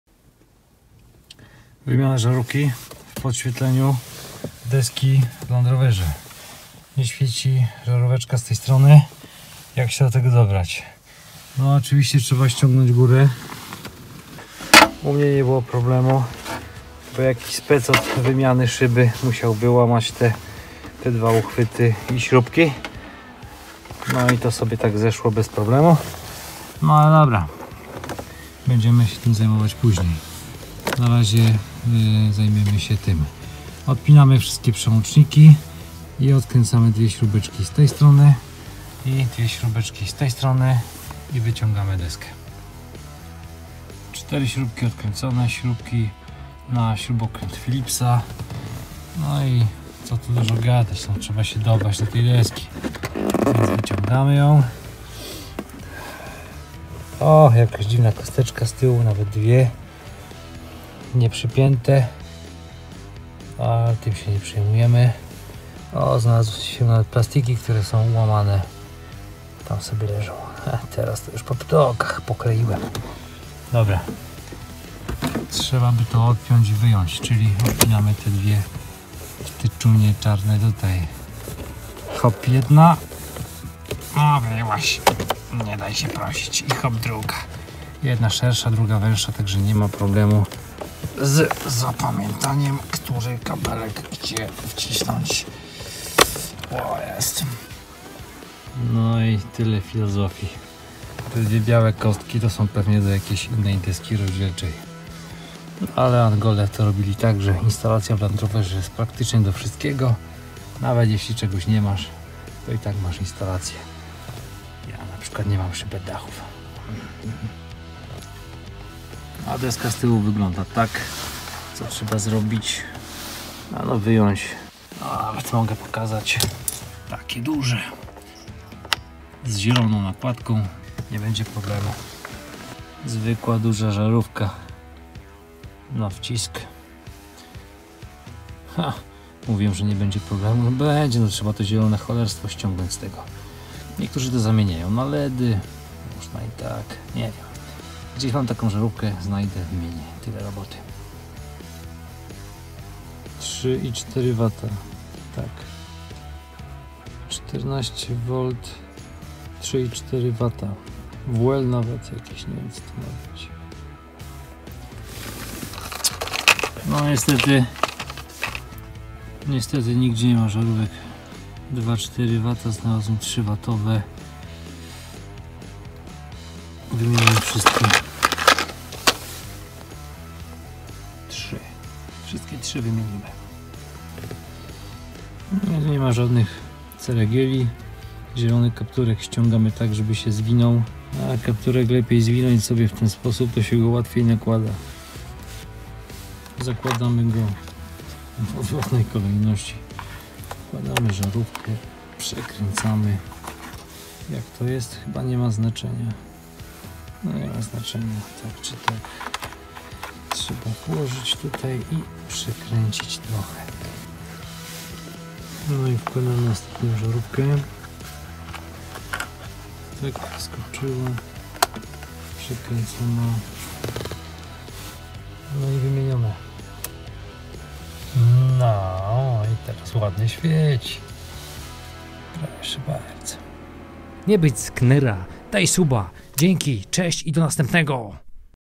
Land Rover D2 - wymiana żarówki podświetlającej zegary
Dziś technicznie.
Wymienimy żarówkę 12V 3W W3W w podświetleniu deski z zegarami w Landrover Discovery 2.